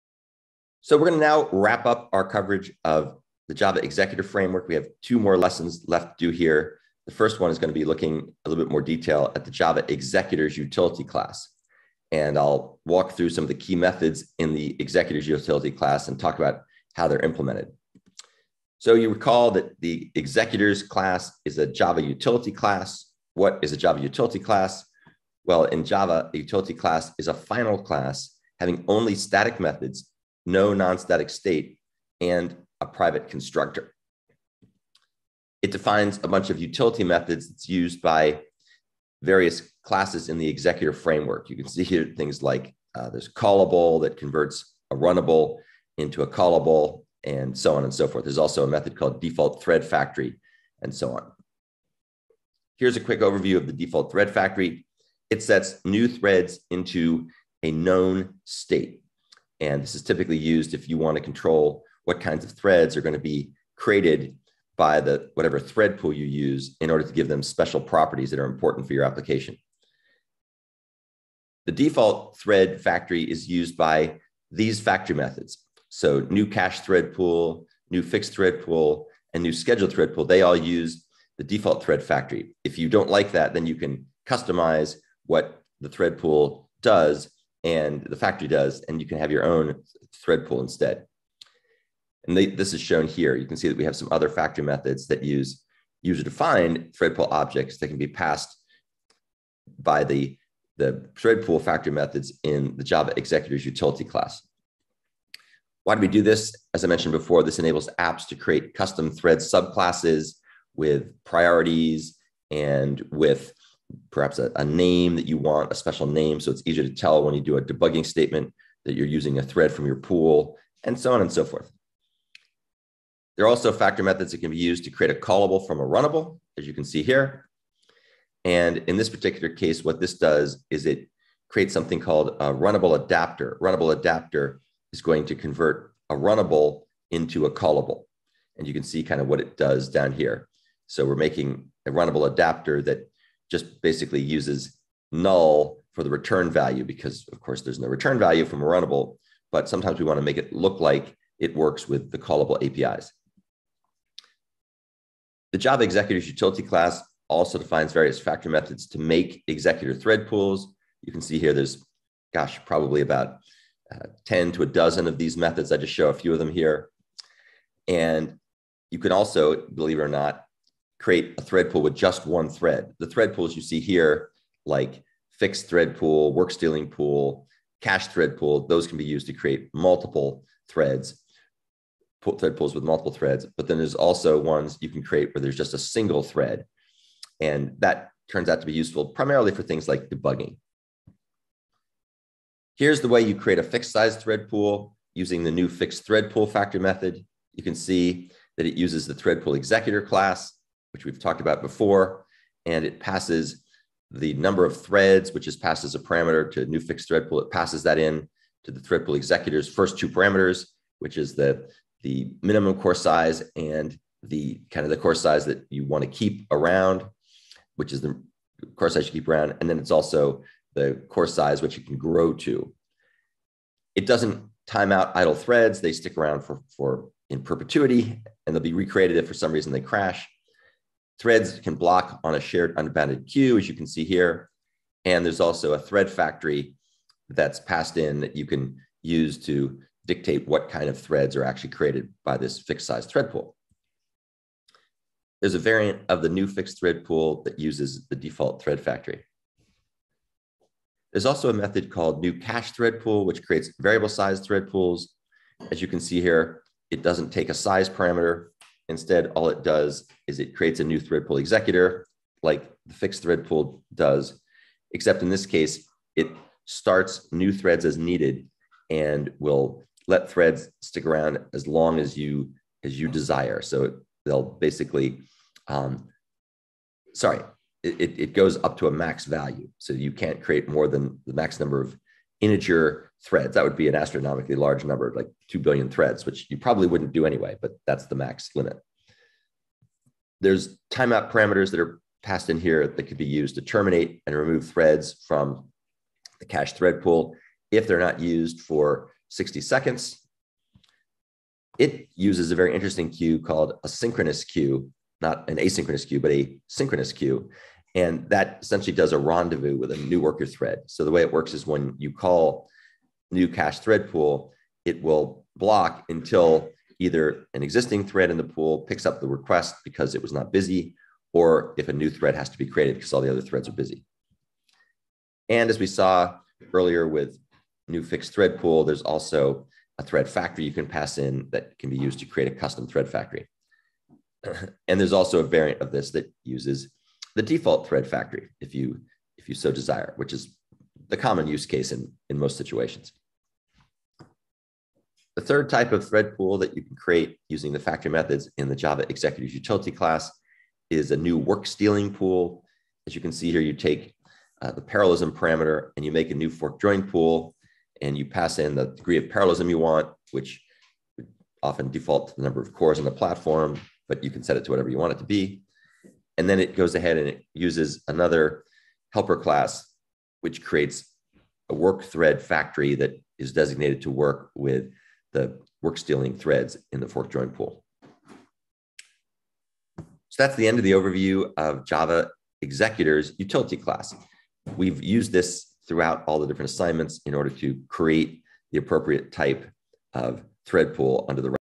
The Java Executors Utility Class
This video describes the implementation of key methods in the Java Executorsutility class.